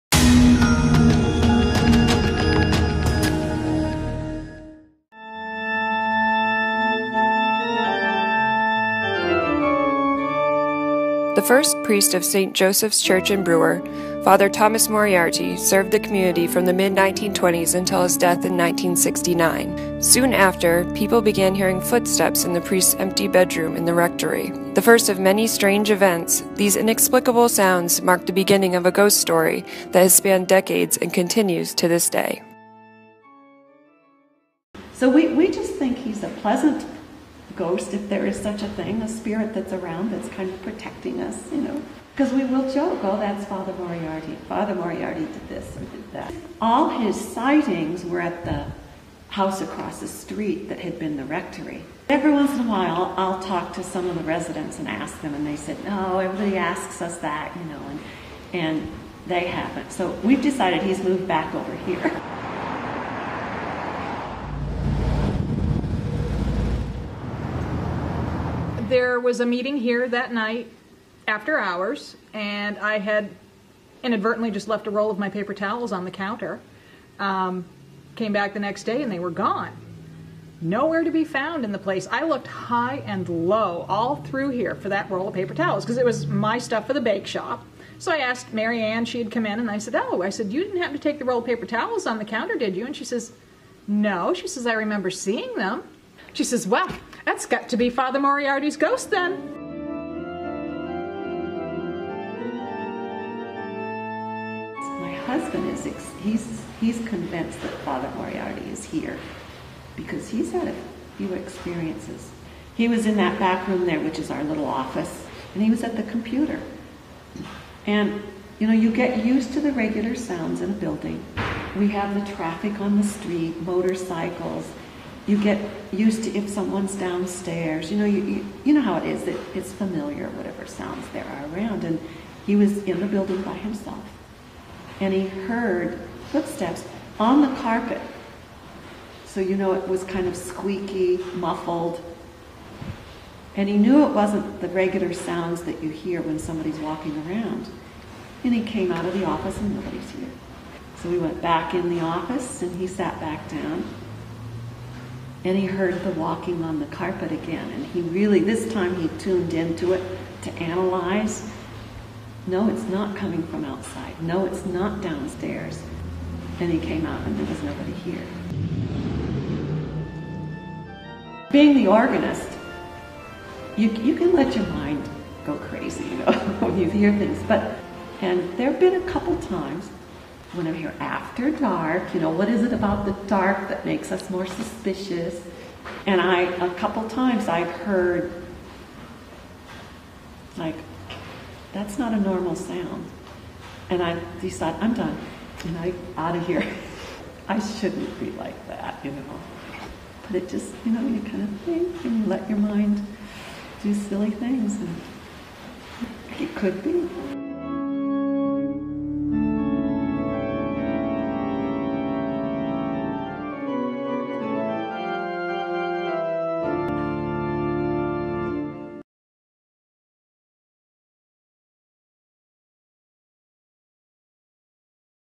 The ghost story of Father Moriarty endures in Brewer church
The first priest of Saint Joseph's Church in Brewer, Father Thomas Moriarty  served the community from the mid-1920s until his death in 1969. Soon after he passed away, people reported hearing footsteps in the priest's empty bedroom in the rectory. The first of many strange events, these inexplicable sounds marked the beginning of a ghost story that has spanned decades and continues to this day.